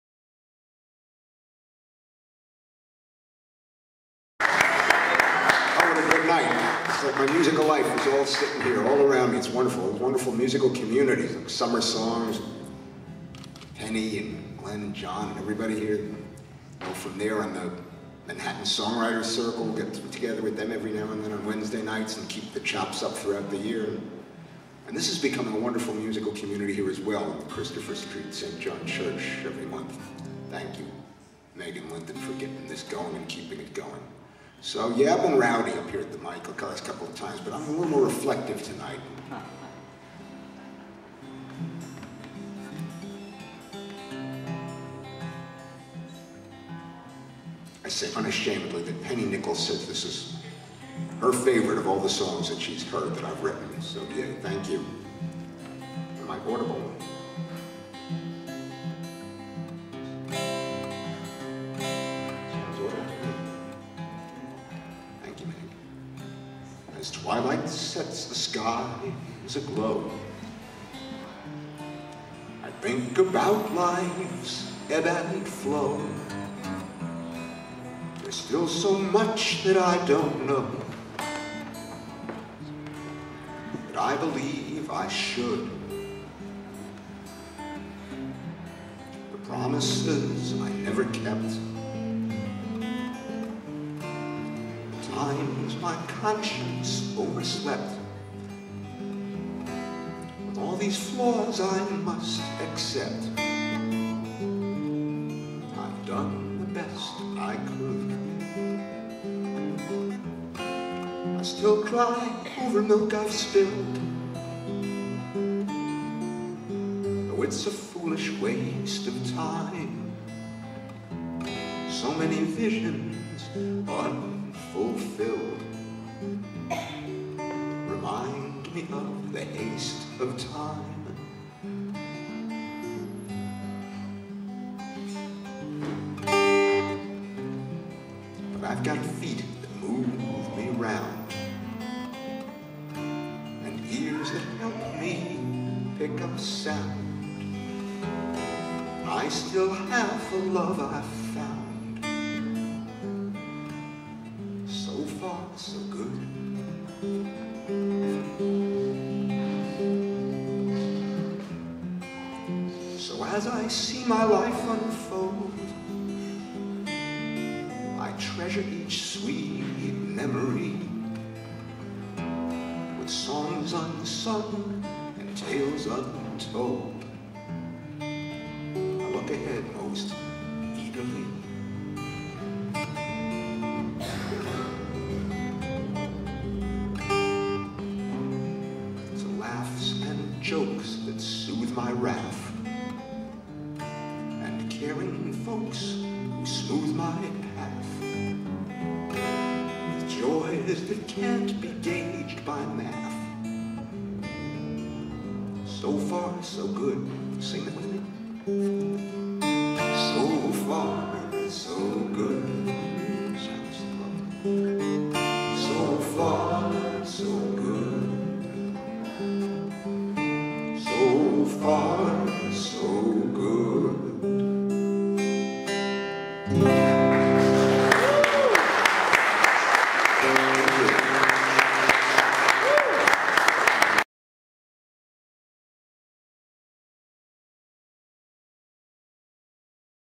Toby Fagenson
Toby Fagenson performs on Open Mic Night at The Christopher Street Coffeehouse in New York's Greenwich Village